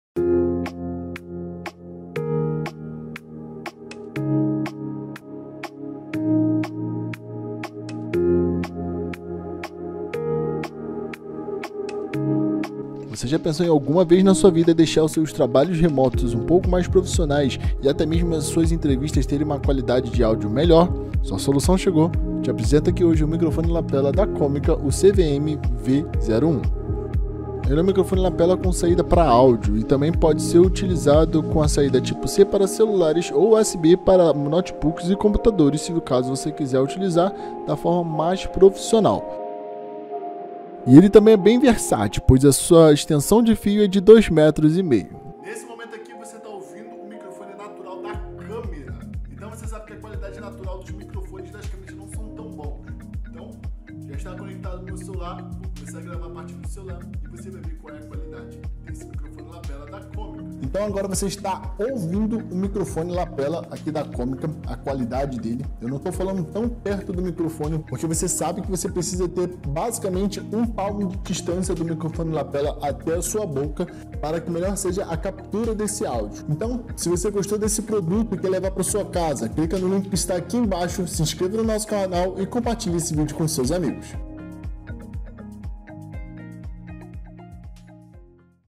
Microfone de Lapela Comica CVM-V01.USB Condensador Omnidirecional para Smartphones (USB-C)
O Microfone de Lapela Comica CVM-V01.USB Condensador Omnidirecional para Smartphones (USB-C) é microfone lavalier clip-on projetado para proporcionar um som de alta qualidade para vlogs, vídeos do YouTube, entrevistas e muito mais. Além de ser amplamente compatível com smartphones de entrada USB tipo C, ele vem acompanhado de um adaptador USB-C para USB-A permitindo assim a conexão com computadores, notebooks e muito mais.

Este Microfone de Lapela possui um clipe de fixação que se prende facilmente à roupa, permitindo que a cápsula seja posicionada perto da boca do locutor para um som claro e nítido, é capaz de gravar áudio com nível de superior ao que o microfone embutido de um telefone proporciona. Há também uma entrada de fone de ouvido de 3.5mm para monitoramento em tempo real e comunicação ao vivo.

O que achou do vídeo? Caso tenha alguma duvida deixa nos comentários, teremos o prazer de ajudar.

Obrigado eMania Foto e Vídeo